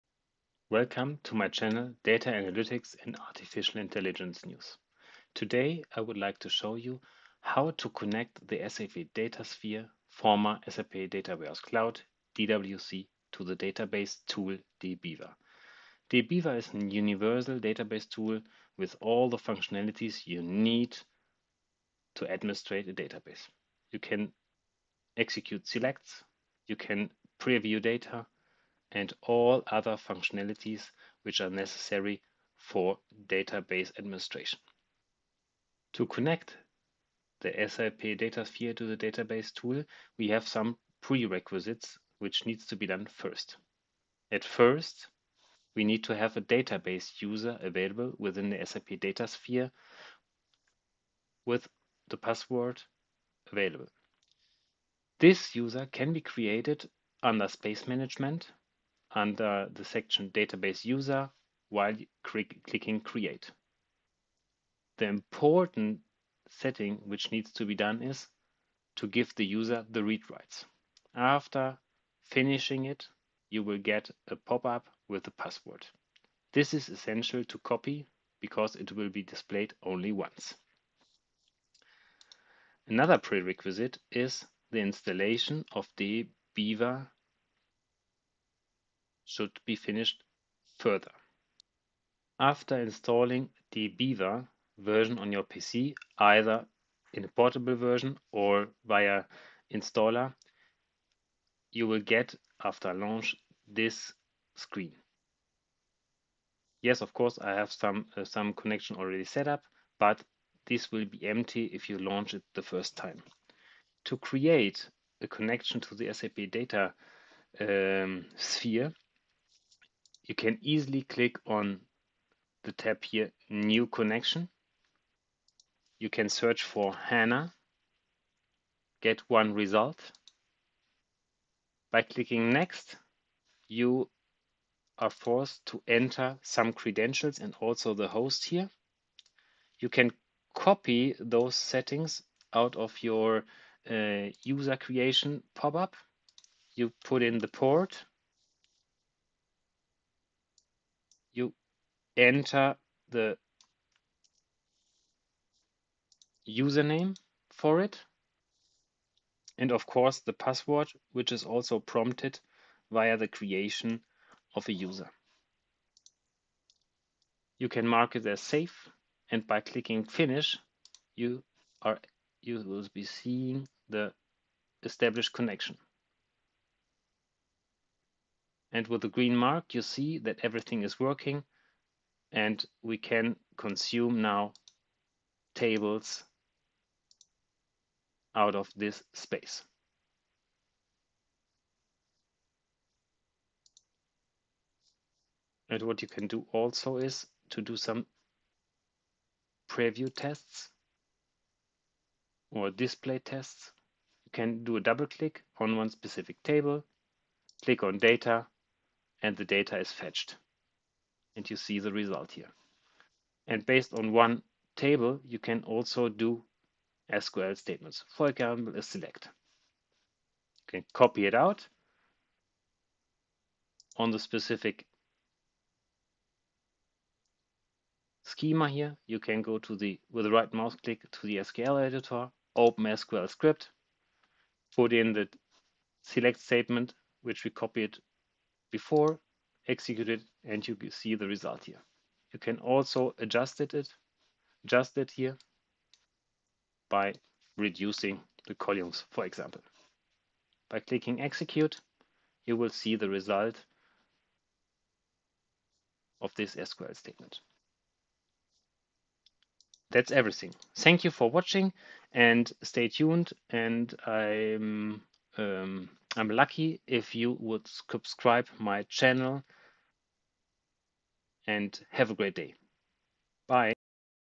Connect SAP Datasphere to the universal database tool DBeaver
In this video I show you how you can connect the SAP Datasphere to the universal database tool DBeaver.

Prerequisites:
- DB User set up in SAP Datasphere
- IP Allowlist maintained in SAP Datasphere